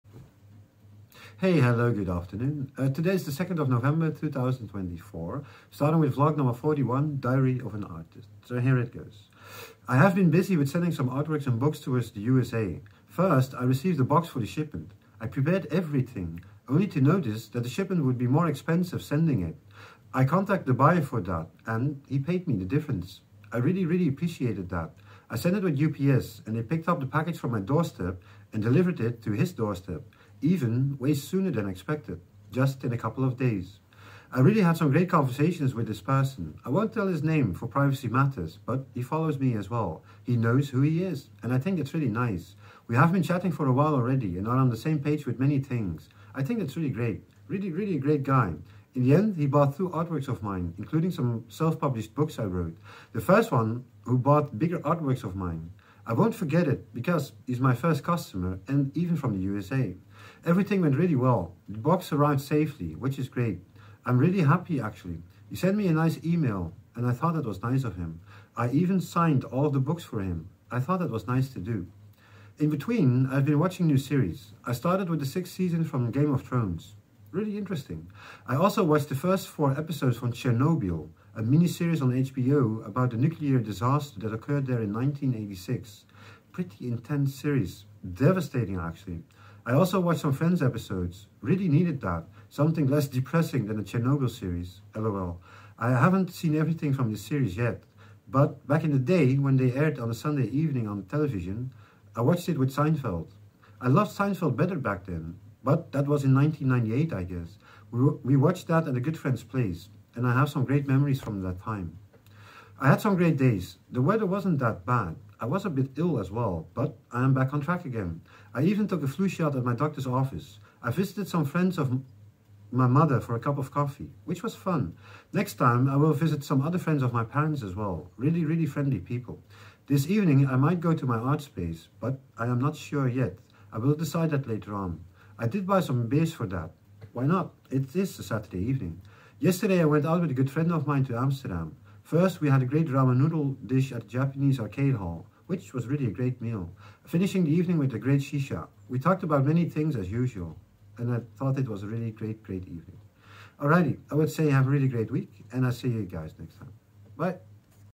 Diary of a schizophrenic artist: 2024: vlog 41
Sending some artworks to the USA. Watching series. Had an easy week.